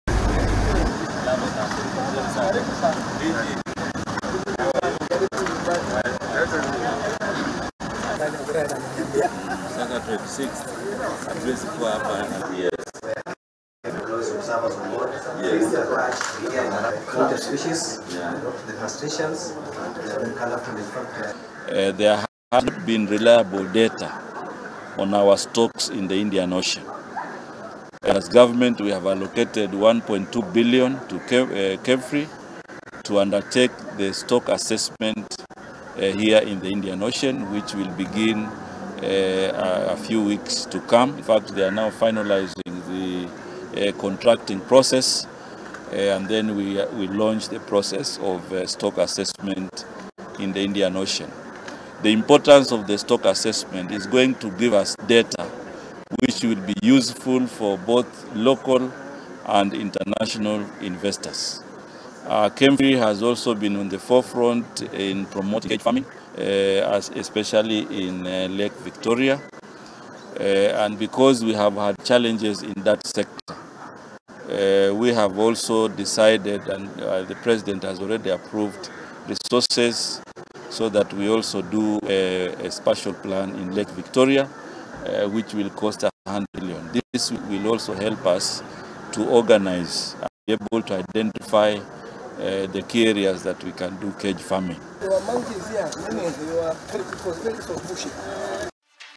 THIRIKARI KUHUGUKIRA UTONGA WA MAI-INI
Minista wa wenji wa kware, utongo na ukui wa mai-ini  salimi mvury  niaguti thirikari niihugukiire gwagirithia utonga wa  mai-ini  nigwo kuguna akenya kuhetukira  guthukira  cukuru wa kenya marine and fisheries institute (kmfri) wiingiririki wa kugania data ya thamaki iria itigarite iriana ria kihindi.
 Salimiu augeti thirikari niiigite keheri-ini birioni 1.2 nigwo kuhutithia mubango uyu.